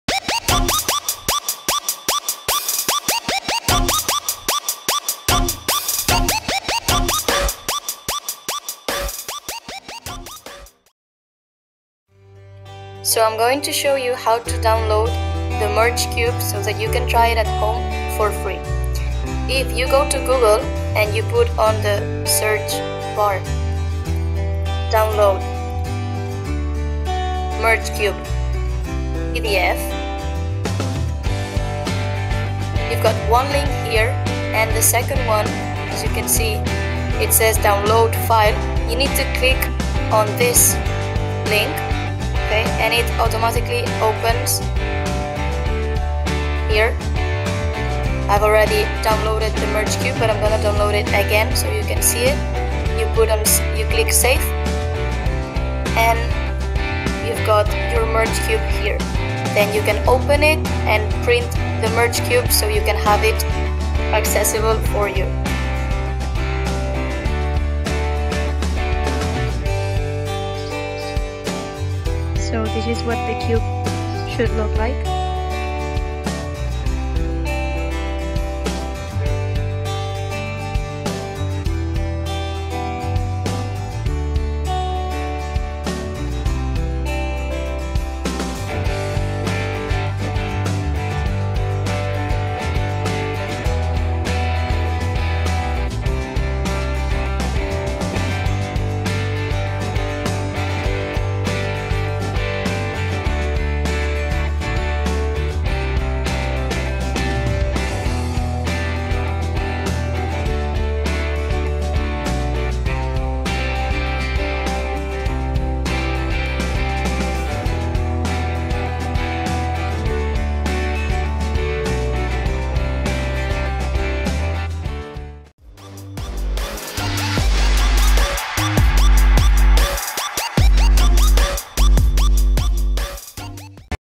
COOL APPS - MERGE CUBE
In today's video... Want to hold Coronavirus in your hands?
A Heart?
You can do all of this and so much more with the merge cube!
Object viewer is just one of the many apps that you can use!